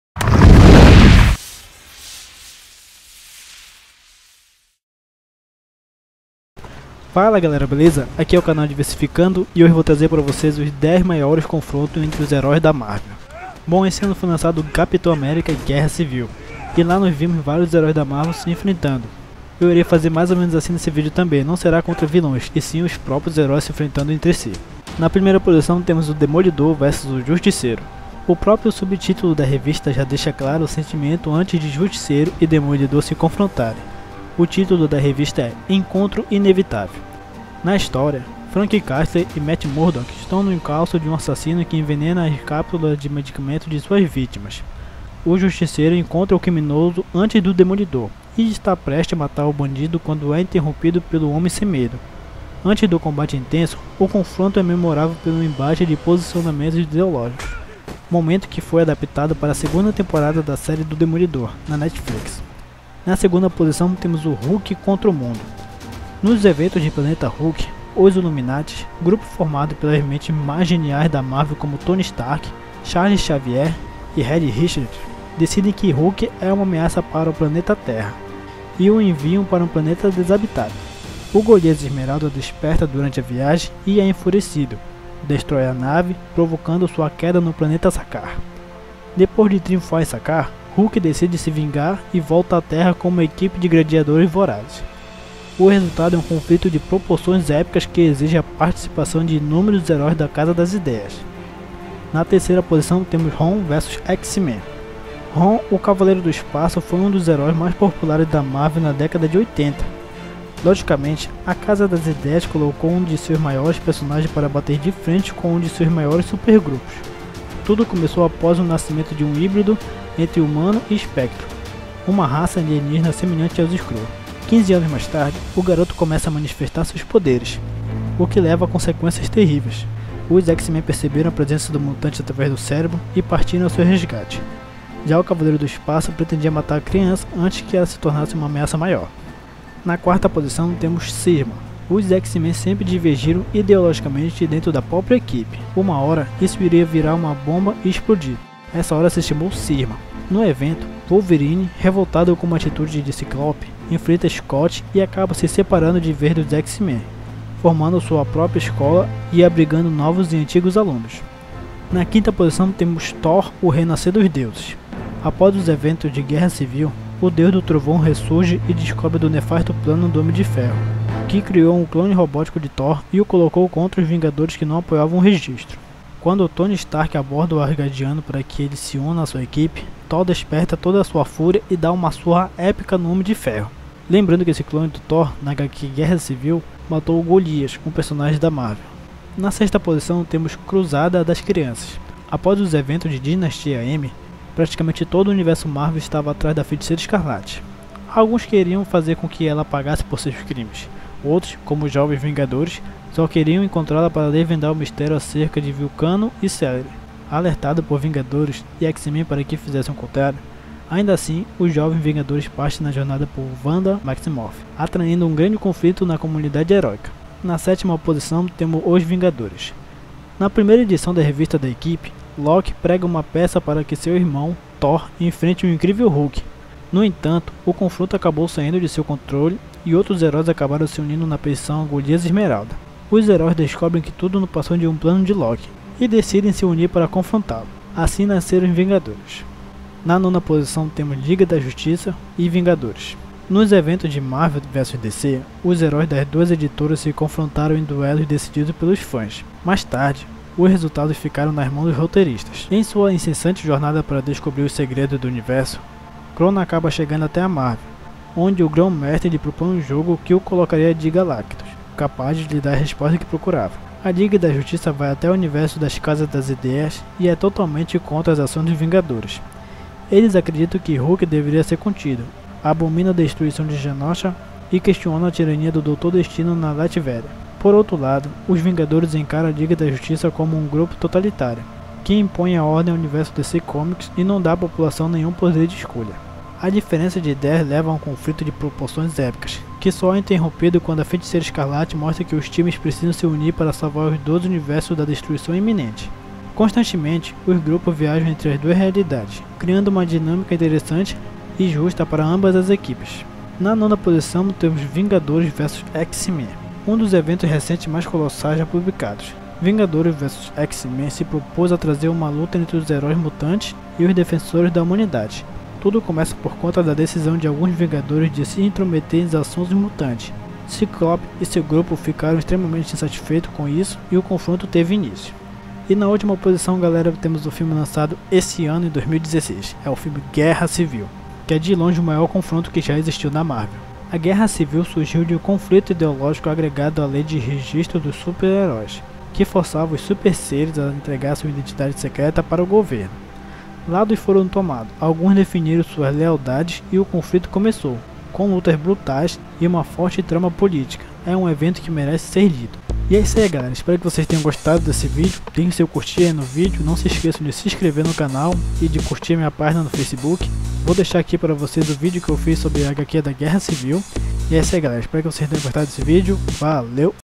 Os 10 maiores confrontos entre hérois da Marvel #14
Fala galera este é o meu video falando sobre os maiors confrontos já ocorridos no universo marvel, espero que gostem.
Referência.
Os 10 maiores confrontos entre hérois da Marvel